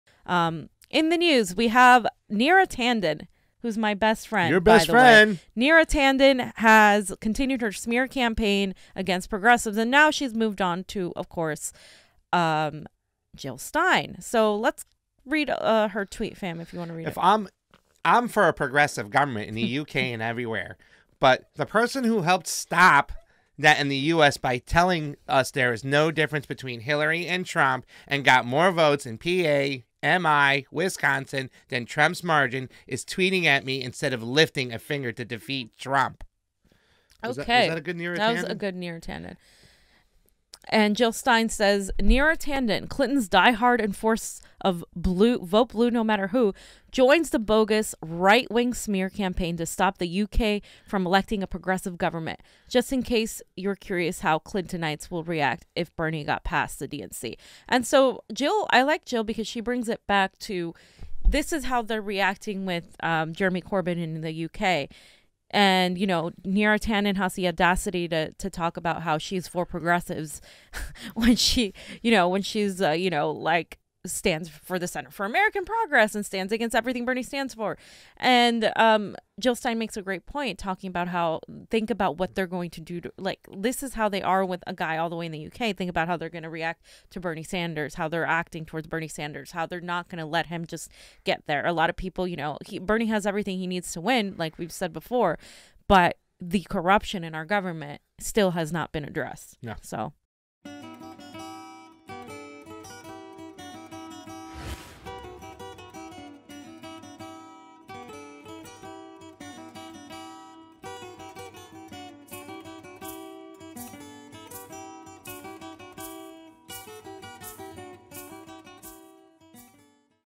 Jill Stein savage against Neera Tanden
Clipped from 12-11-19 Livestream show

Jill Stein responds to Neera Tanden's tweets where Neera joined the right-wing movement in the UK smearing Jeremy Corbyn.
EVERYBODY SHARE SHARE SHARE. SPREAD THE TRUTH.
Keep Up With THE CONVO COUCH 🛋